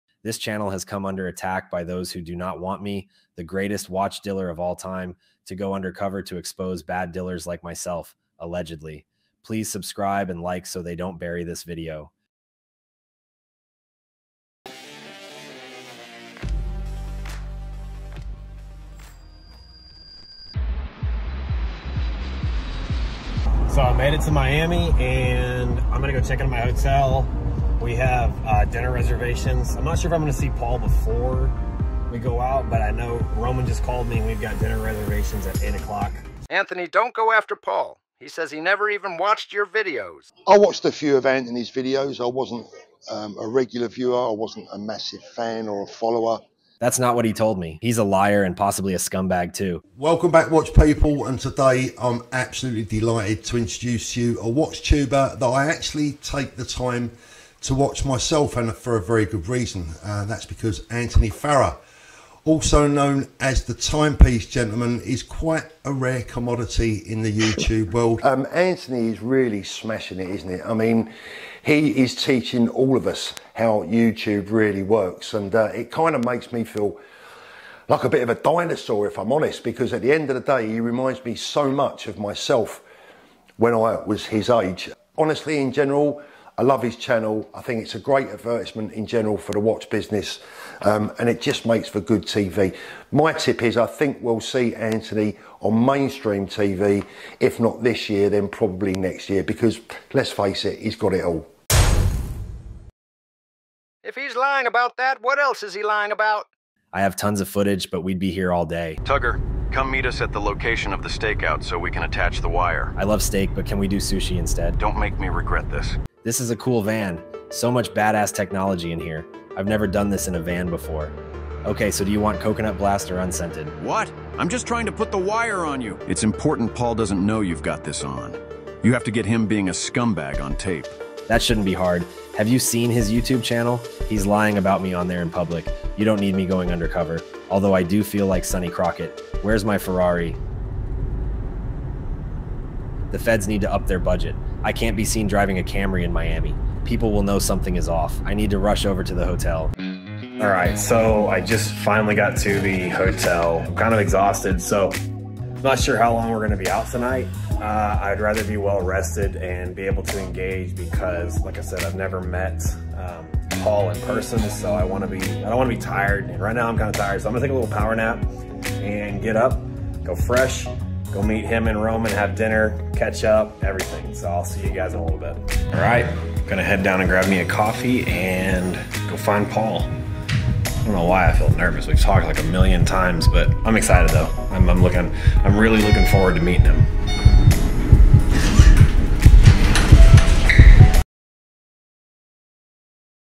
Watch Wars Escalate: Anthony Farrer's Miami Mission Against Paul Thorpe Unveiled!
Anthony lands in Miami, Florida to go undercover and after watch dealer Paul Thorpe in this episode of the Parody Timepiece Gentleman Series.

This alternate timeline version of The Timepiece Gentleman is a work of pure fiction and starts from the day Anthony Farrer posted the $5 Million Dollar Debt video. All characters appearing in this work are
fictitious. Any resemblance to real persons, living or dead, is purely coincidental.